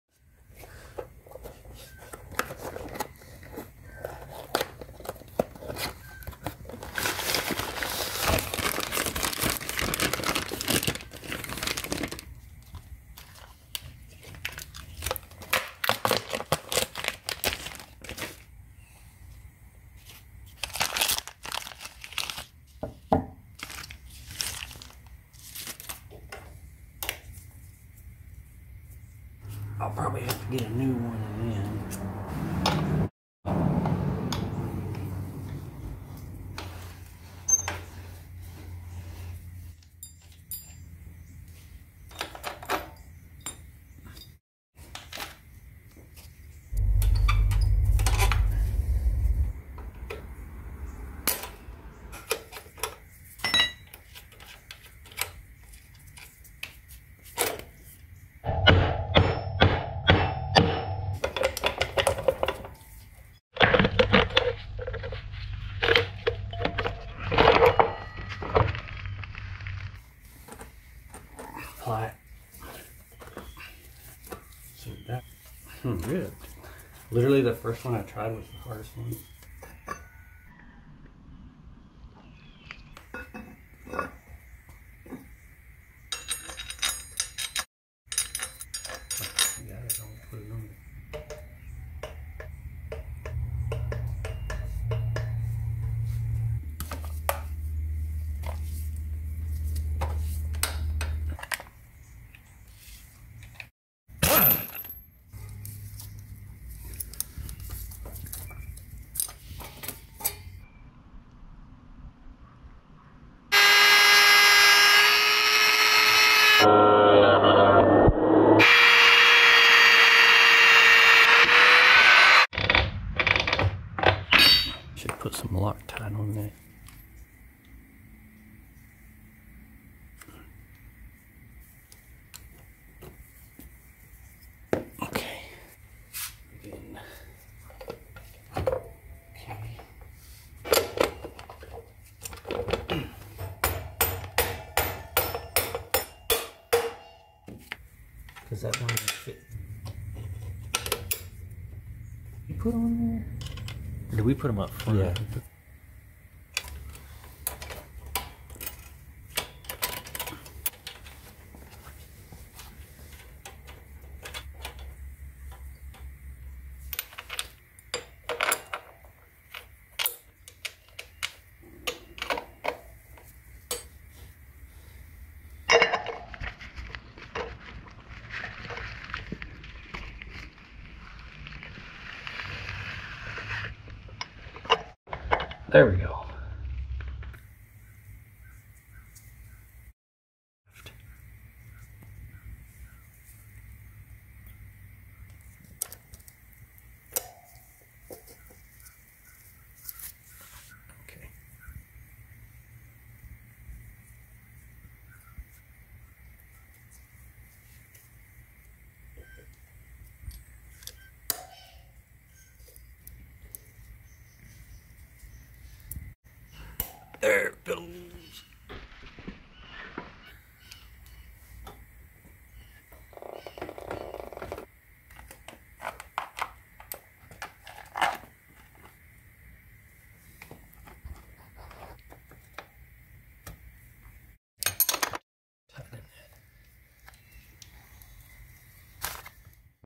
DR 200 New Sprockets and Chain
We install a new chain and set of sprockets on the DR 200